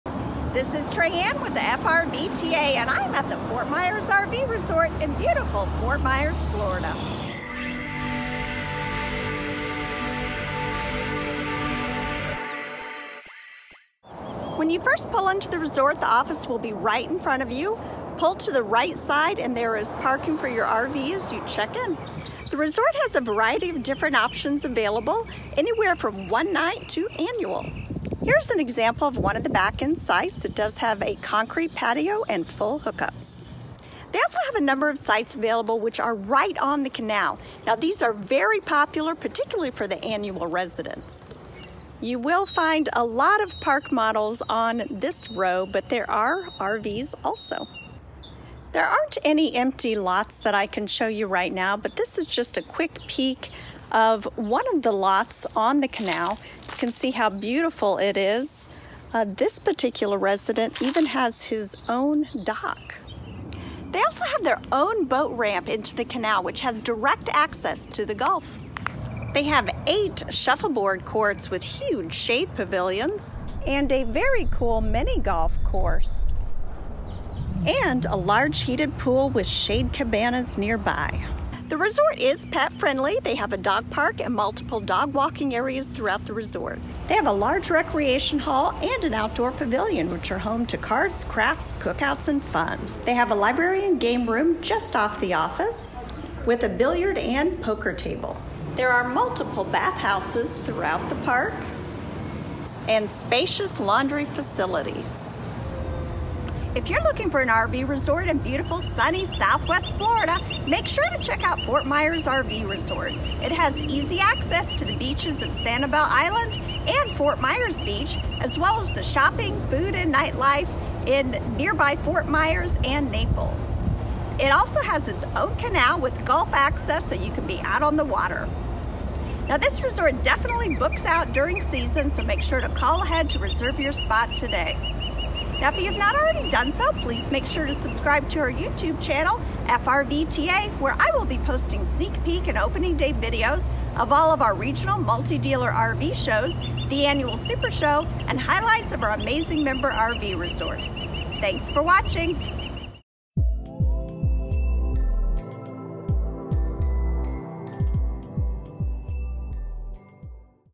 Ft Myers RV Resort
Ft Myers RV Resort is located on the gorgeous Gulf Coast of Southwest Florida with it's own dock and water access to the gulf. It is a short drive to the white sand beaches of Fort Myers Beach and Sanibel Island with plenty of restaurants and shopping nearby.